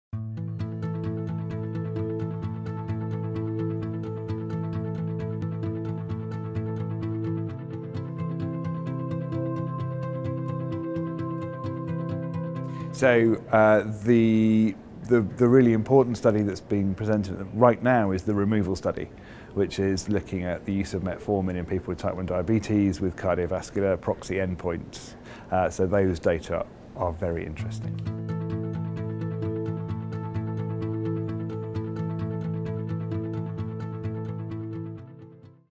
ADA 2017 Highlights – Nick Oliver
Nick Oliver, Wynn Professor of Metabolism Imperial College, London, UK – discusses which presentations and/or data highlighted at the ADA 2017 interested him the most.